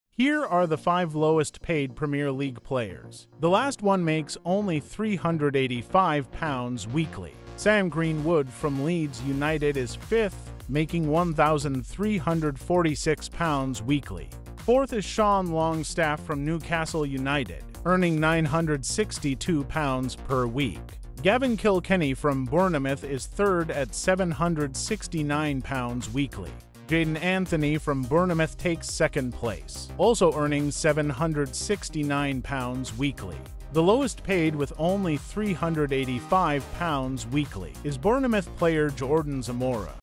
5 Lowest Paid Premier League Players
📉 Unveiling the Premier League's Lowest-Paid Football Players! 💸

Meet the bottom five earners in the Premier League:

Join us as we explore the challenges these players face both on and off the field. From Zamora's £385 weekly wage to Greenwood's £1,346, discover the stories behind the numbers in the world of football finance.